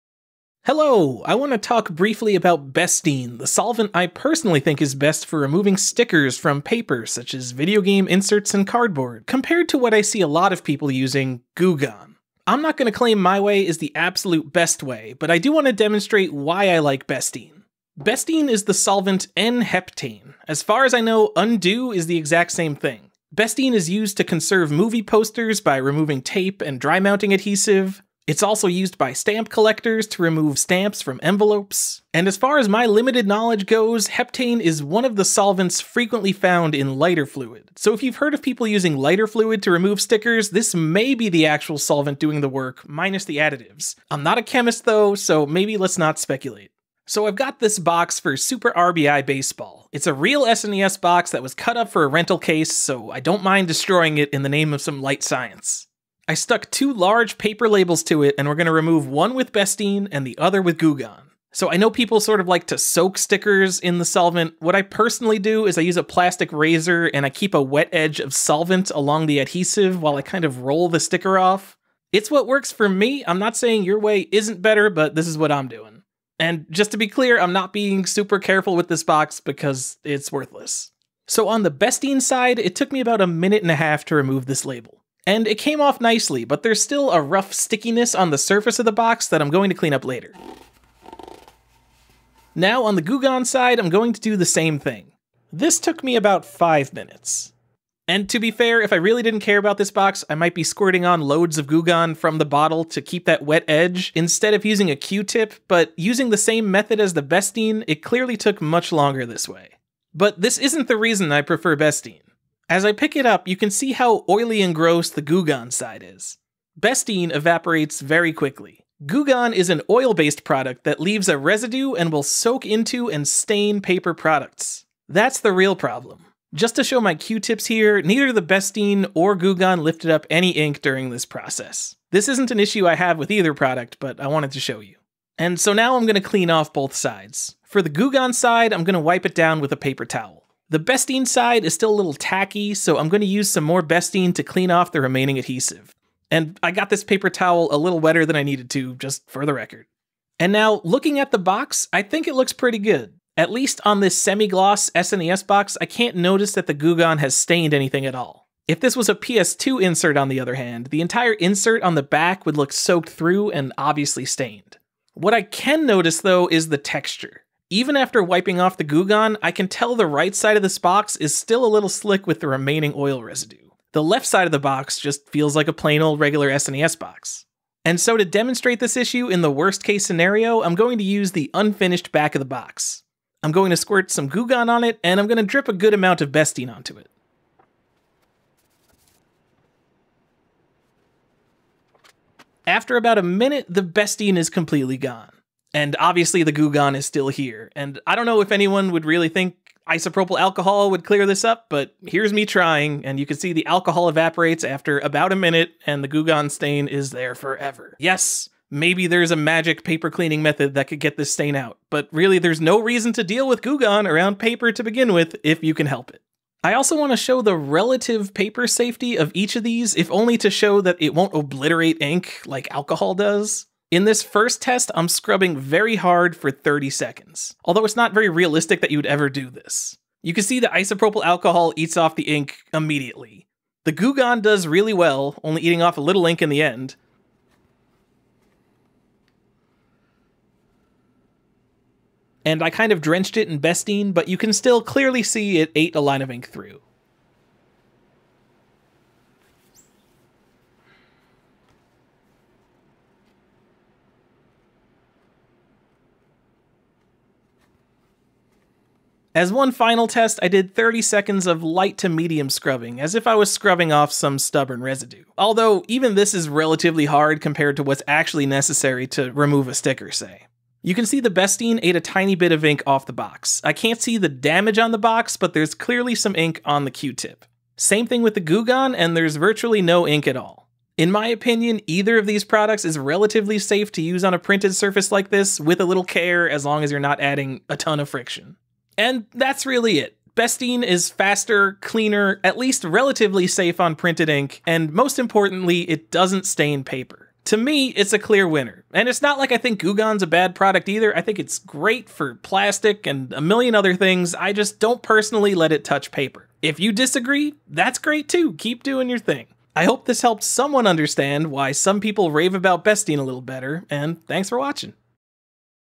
Bestine vs. Goo Gone (for video games)
A short video about why I recommend Bestine for removing stickers from video games in most cases. Not mentioned was that Goo Gone is a safer product than Bestine (in terms of human health), which may be a consideration as well. This is just my opinion so if you're using brasso and steel wool to rub off stickers, keep up the good work buddy.